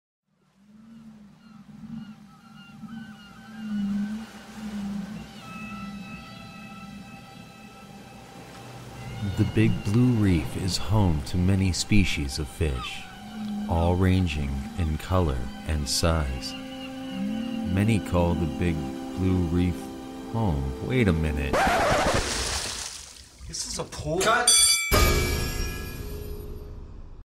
Derek's Underwater Stuff Ep .1 The Big Blue Reef
From DDCC it's the first episode of Derek's Underwater Stuff. We start at The Big Blue Reef!